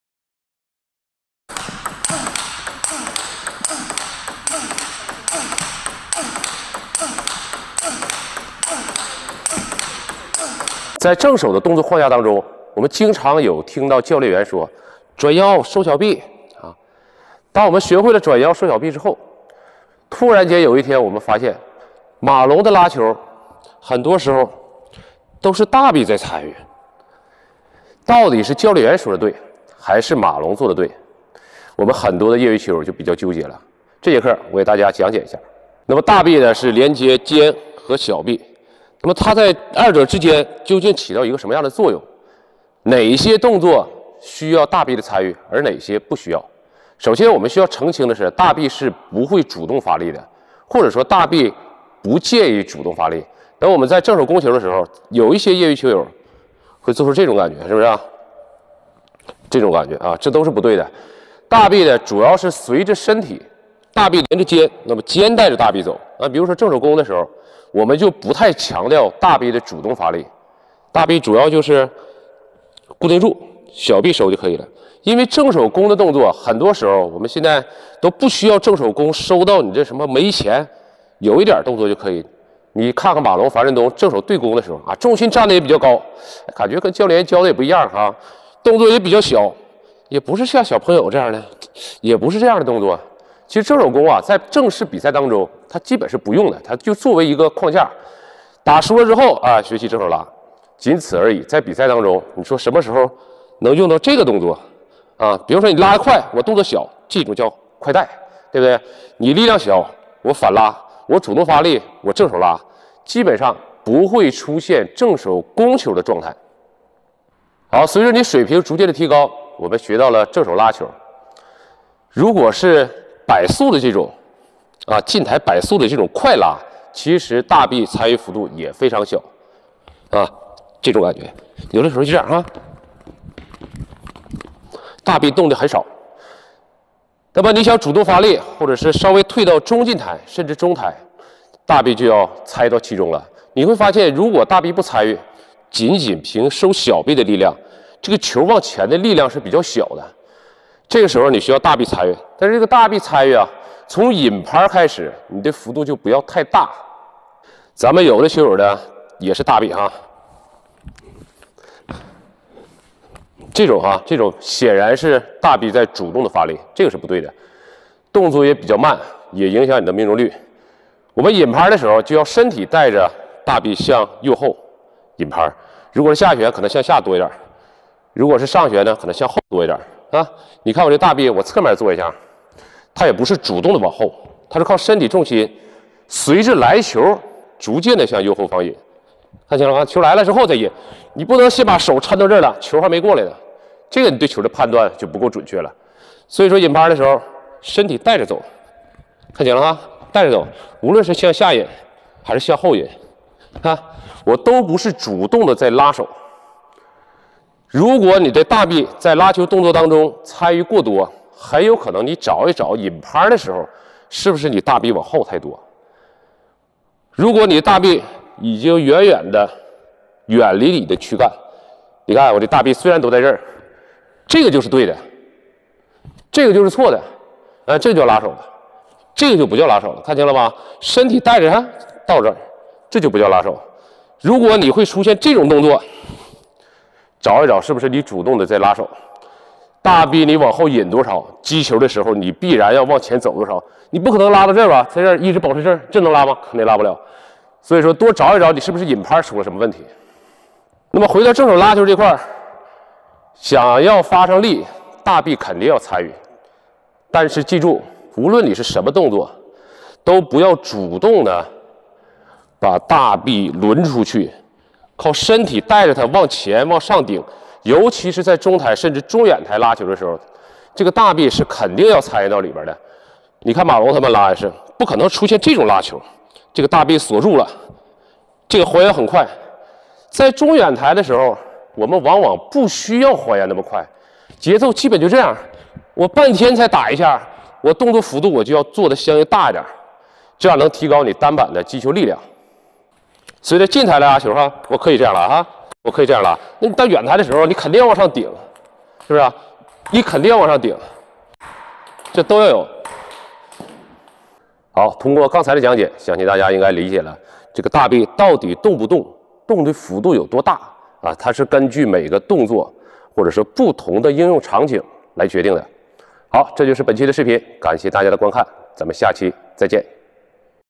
乒乓球技巧讲解，正手击球框架需要大臂参与吗？
大家好！这里是【小宇老师】YouTube官方频道😎
每天不定时我大家带来不一样的“乒乓球教学”
感谢那些：
                   支持我的人

小宇老师 教学 乒乓球# 请注意，本视频是合法的体育赛事内容，与暴力无关。